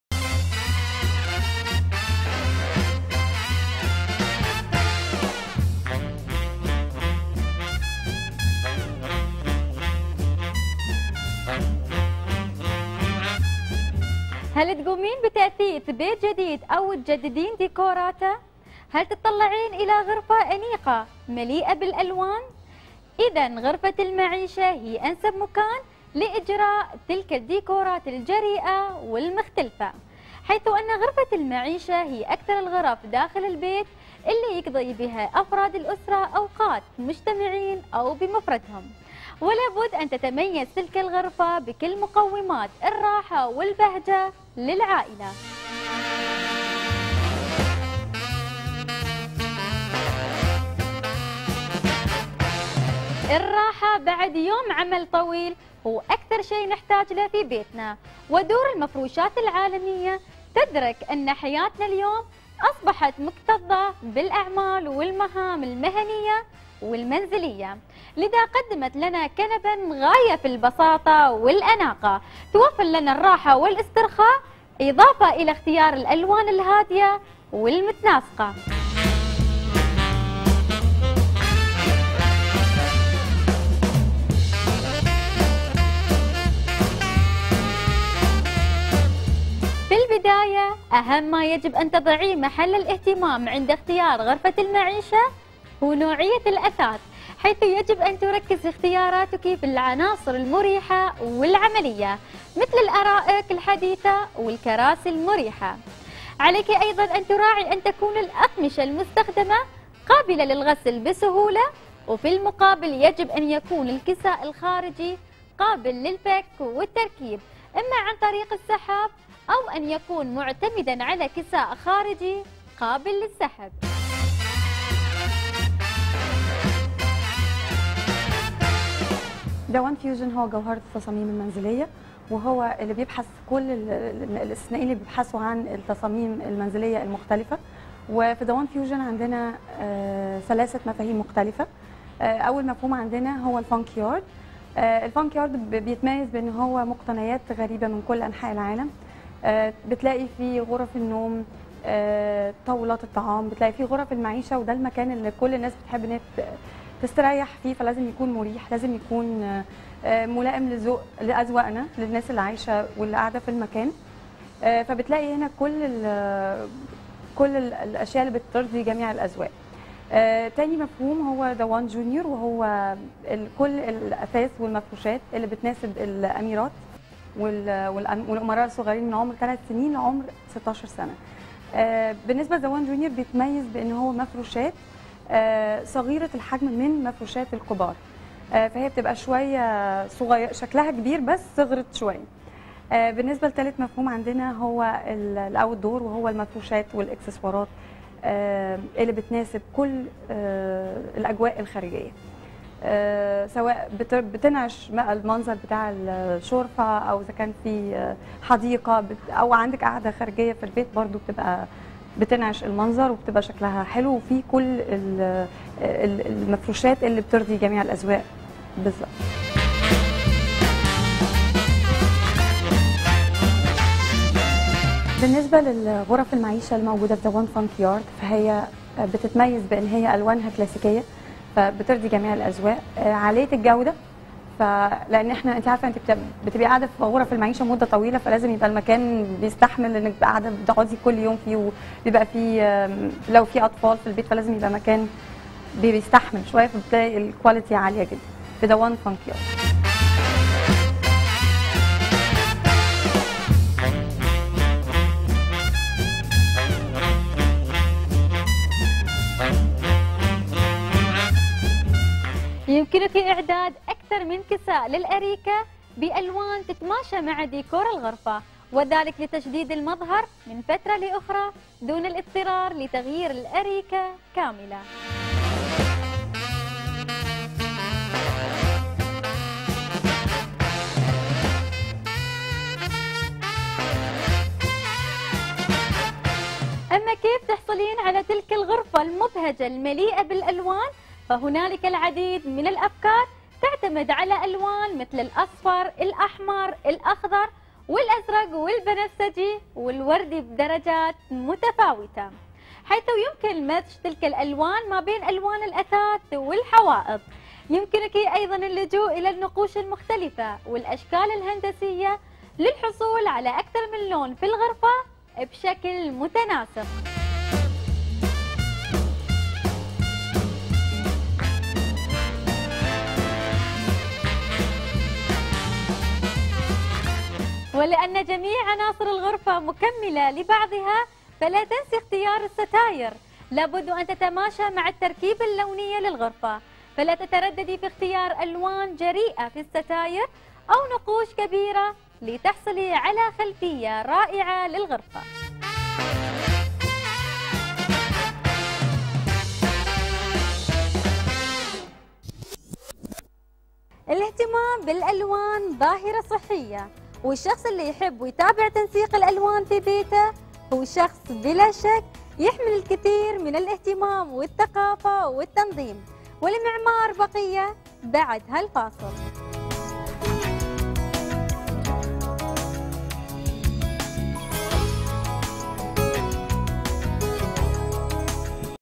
THE One Fusion interview on Sama Dubai TV, May 2014
And now, one for all our Arabic fans! Everything you never knew you wanted to know about THE One FunkYard, Junior and Outdoor... as seen on Sama Dubai TV, filmed at THE One Fusion, Dubai. Good news for our Abu Dhabi, Kuwait and Qatar fans! THE One FunkYard, Junior and Outdoor collections are also available at THE One Khalidiya and THE One Marina Mall Theatres, while THE One Junior store is now open in Lagoona Mall, Doha. Happy shopping!